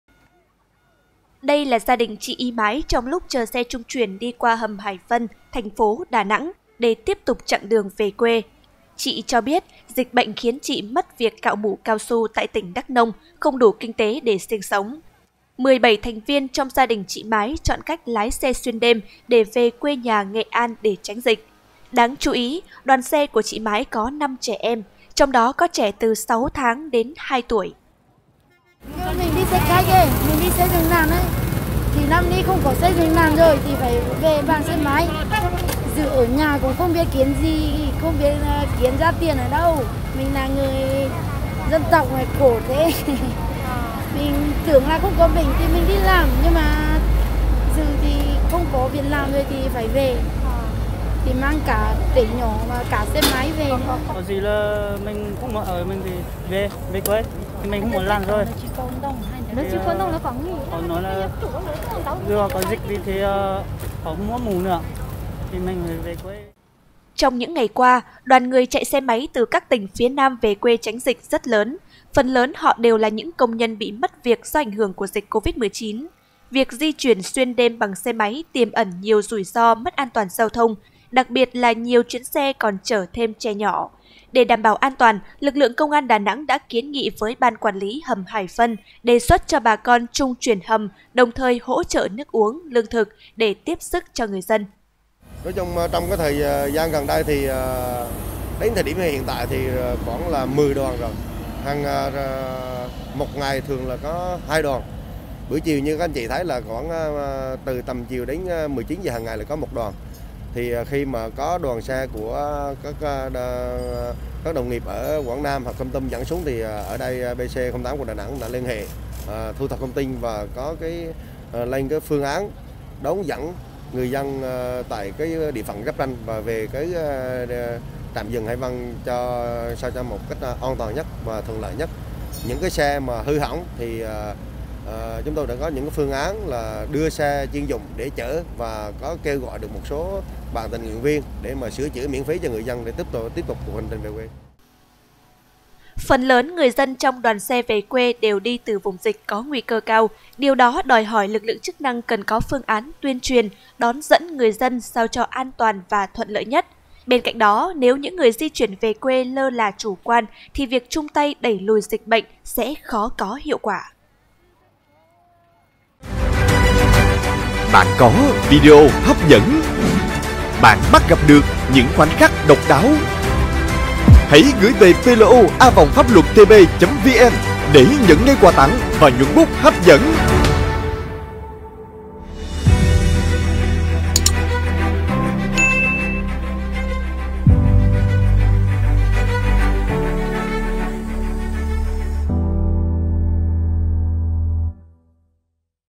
Những 'thiên thần nhỏ' trong hành trình về quê tránh dịch COVID-19 - PLO
Hàng trăm người dân từ các tỉnh phía Nam chạy xe máy vượt hàng ngàn cây số về quê tránh dịch. Trong số đó có không ít trẻ em về cùng bố mẹ.